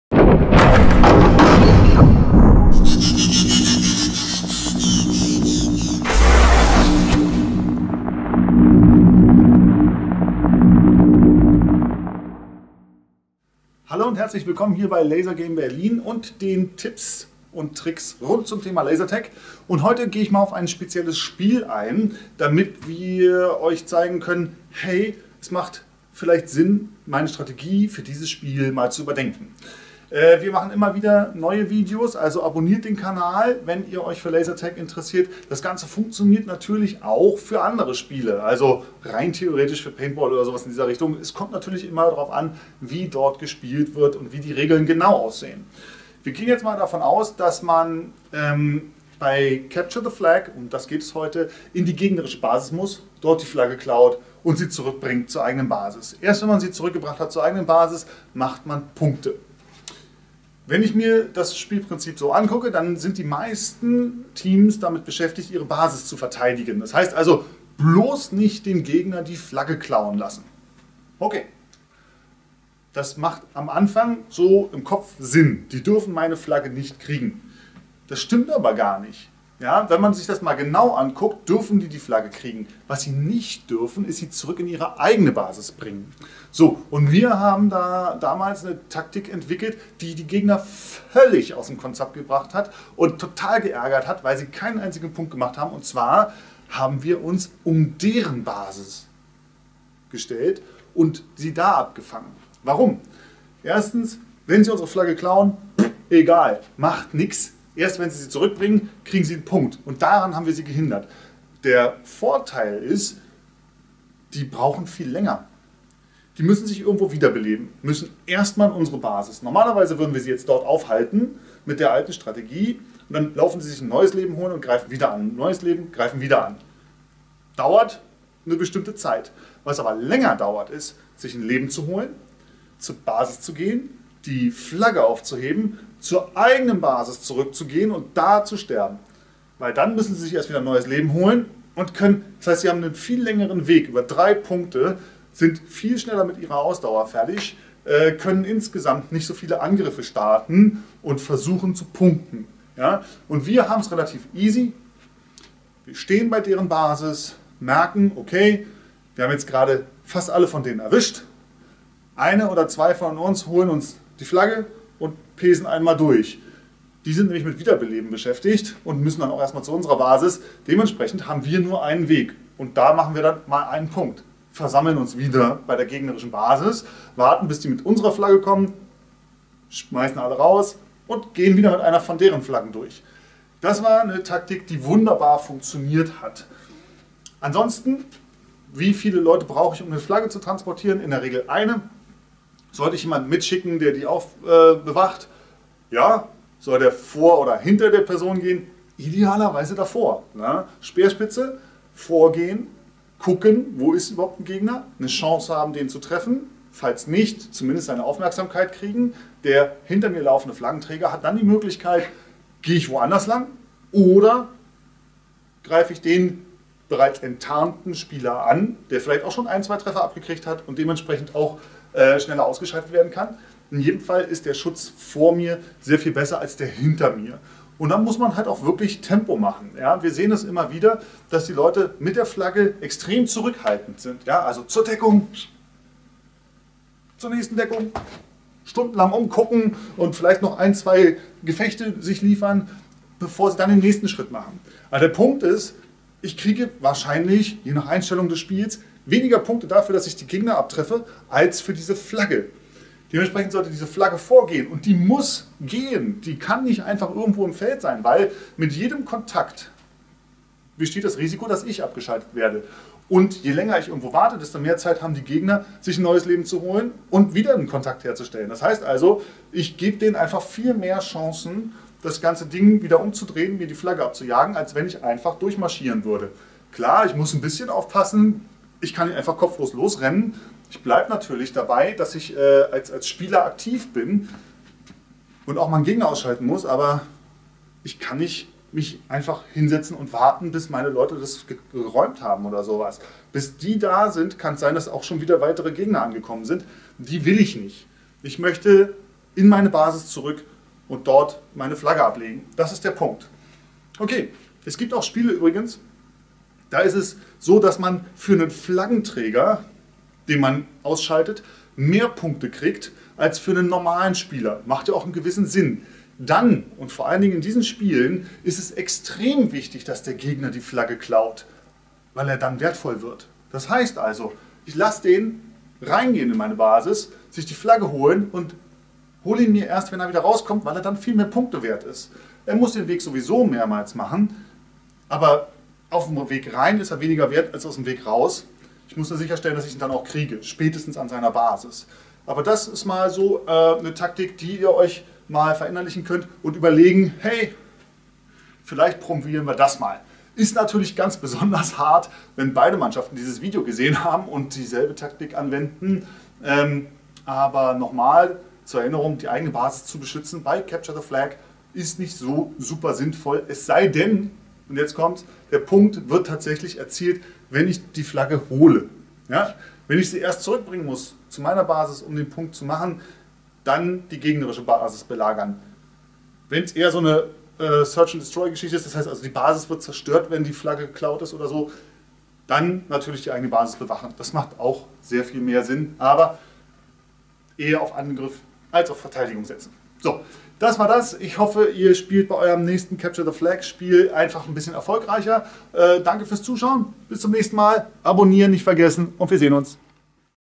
Lasertag Tipps und Tricks 11 - Capture the Flag
Strategien für Capture the Flag - funktioniert wunderbar beim Lasertag und auch bei Computerspielen mit ähnlichen oder gleichen Regeln. Lasertag CTF in Berlin spielen?